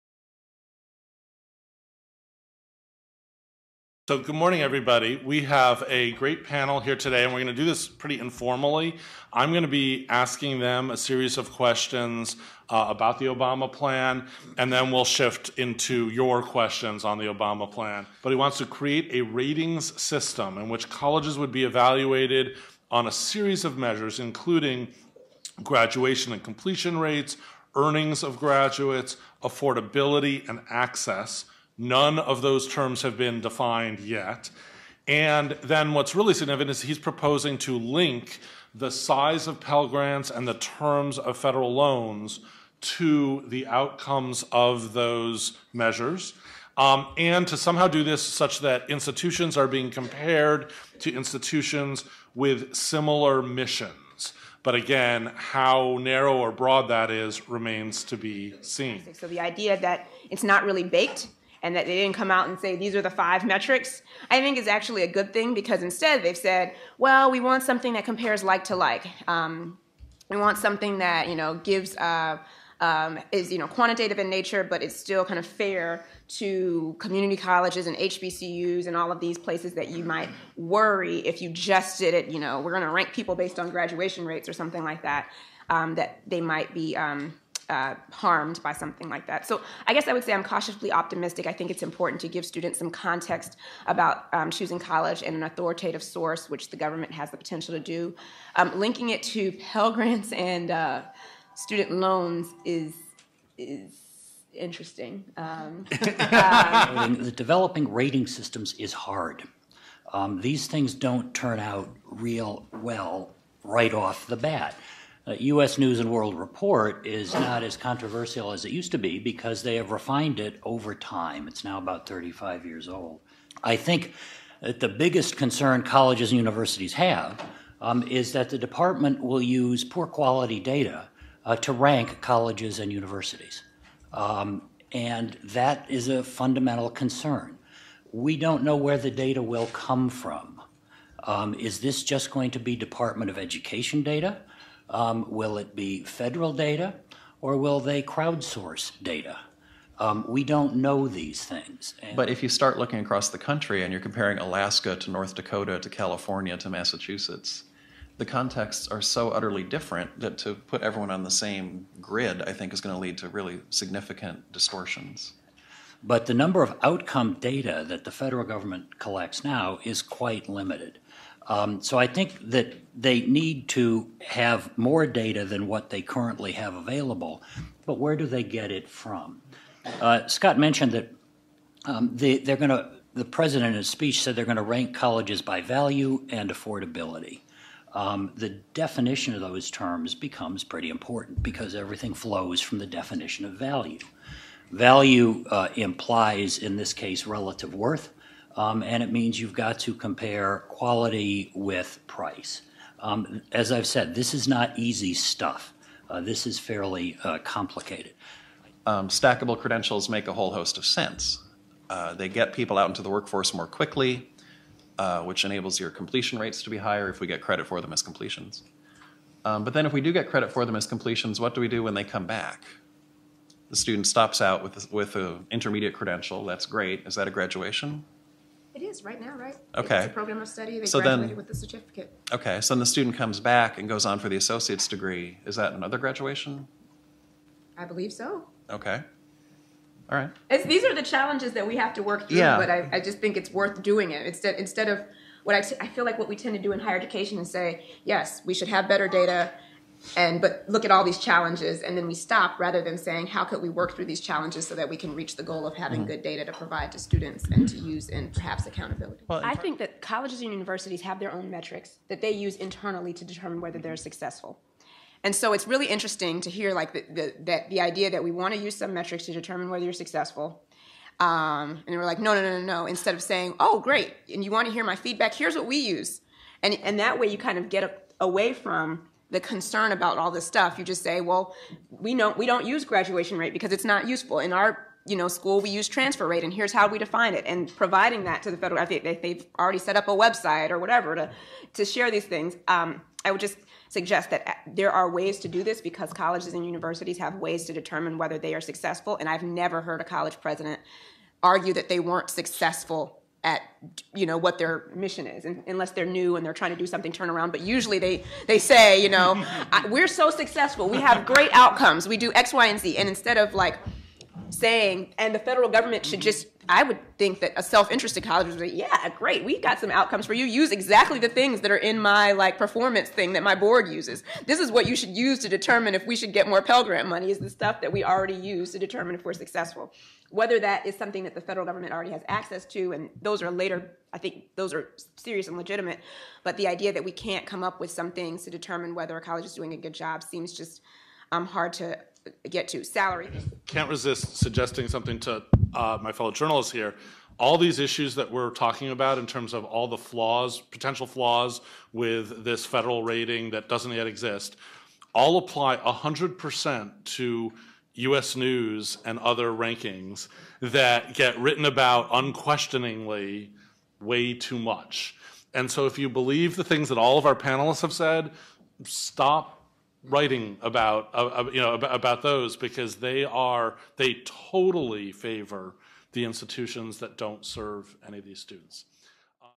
Obama's Proposal: Will Performance Ratings Hurt Student Access?
Recorded at EWA's 2013 Higher Ed seminar, "Guess Who's Coming to Campus: What Demographic Changes Mean for Colleges and Reporters."

Last month, President Obama unveiled an ambitious proposal to reform higher education by tying a college's access to federal financial aid for students to a new set of ratings the government would produce. Would universities, forced to focus more on student outcomes, be less inclined to enroll students from backgrounds that traditionally have been underserved by higher education?

Panelists:
Terry Hartle, Senior Vice President, American Council on Education
Matt Reed, Vice President of Academic Affairs, Holyoke Community College
Zakiya Smith, Strategy Director, Lumina Foundation
Scott Jaschik, Editor and Co-Founder, Inside Higher Ed (moderator)